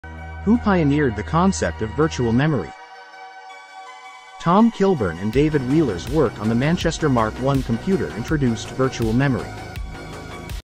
Virtual memory pioneers
Tom Kilburn and David Wheeler's work on the Manchester Mark I computer introduced virtual memory.